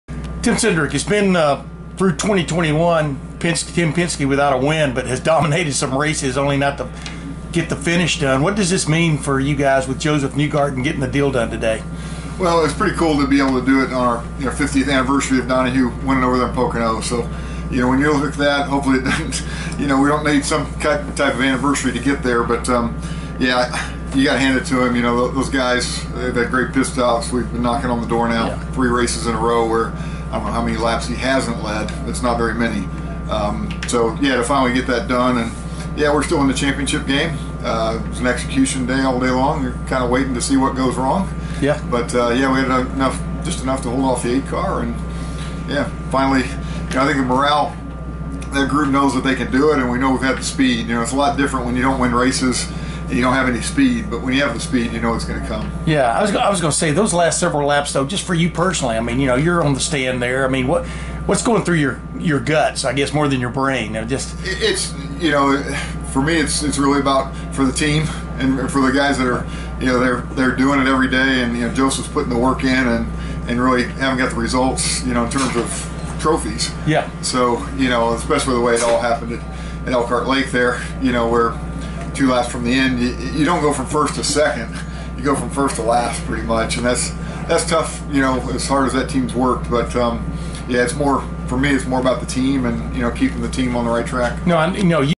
Team Penske president Tim Cindric on Newgarden breaking 2021 ice
The only thing Team Penske hadn't done this IndyCar season was win a race, but otherwise had put in some dominant performances before Josef Newgarden drove from the pole (his third straight) to victory in the July 4 Honda Indy 200 at Mid-Ohio Sports Car Course. Team president Tim Cindric on the icebreaker, and on the win coming on the 50th anniversary weekend of Team Penske's first IndyCar win, by the late Mark Donohue at Pocono in 1971.